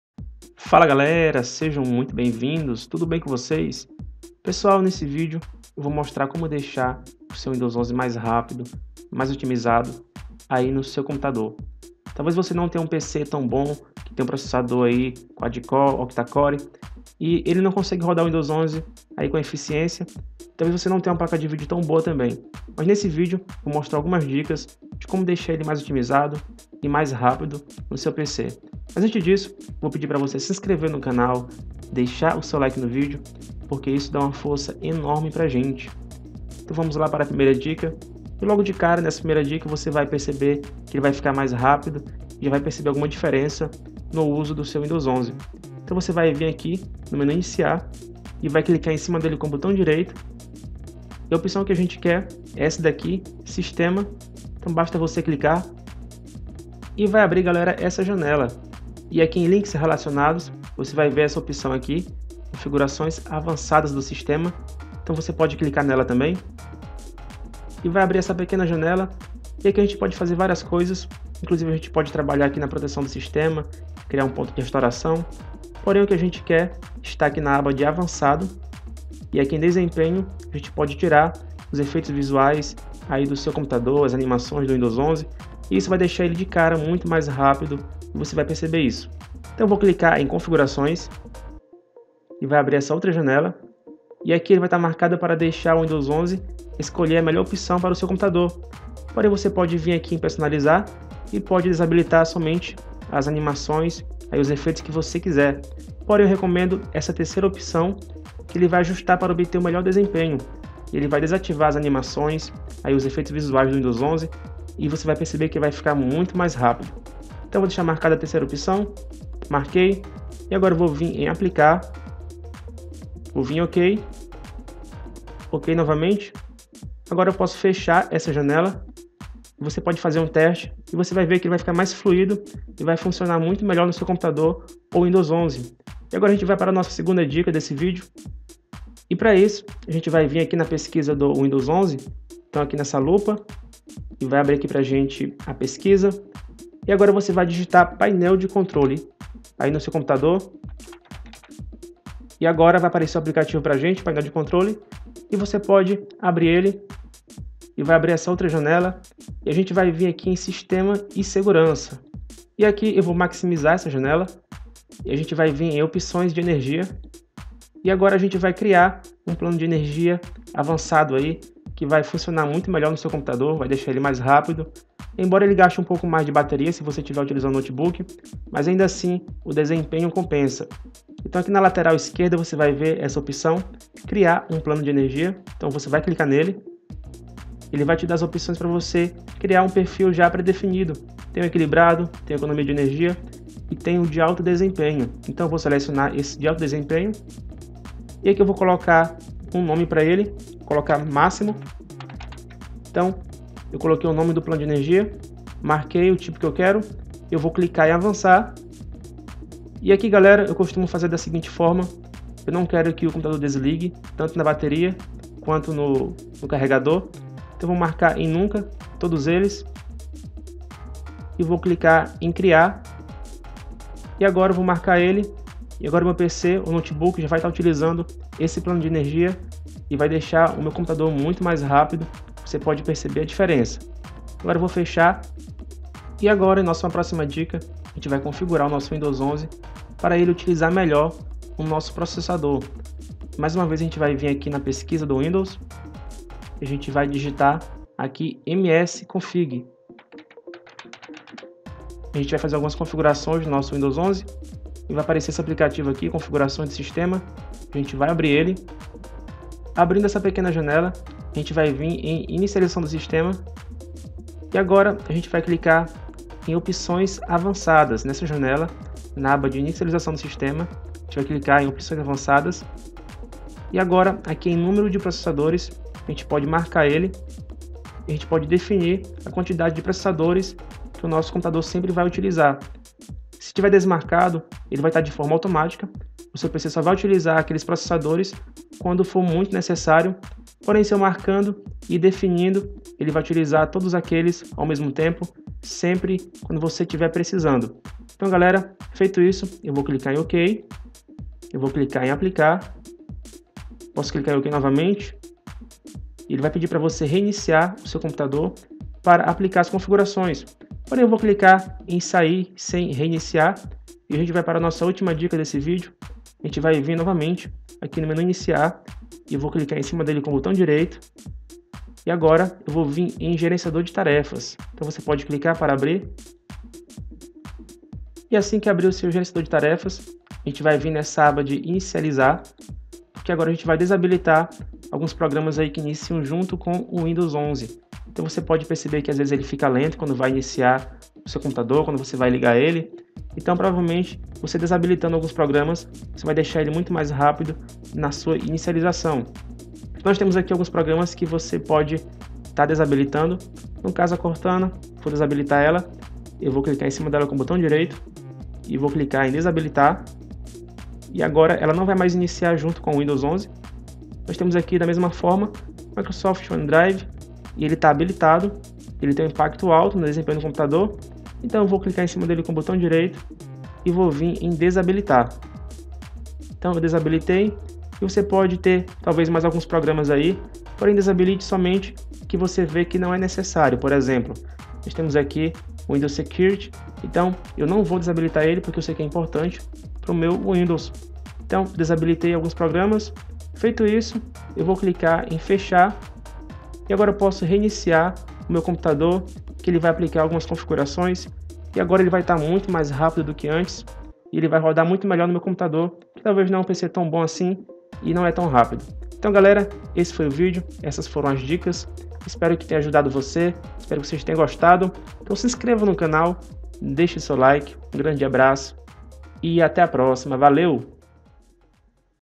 Como Deixar o Windows 11 Mais Rápido e Otimizado
Nesse vídeo mostro passo a passo como deixar o Windows 11 mais rápido e leve, passando dicas que vão melhorar o desempenho em programas e jogos. Tudo isso sem precisar usar programas, apenas configurações e ajustes que deixarão seu Windows 11 otimizado.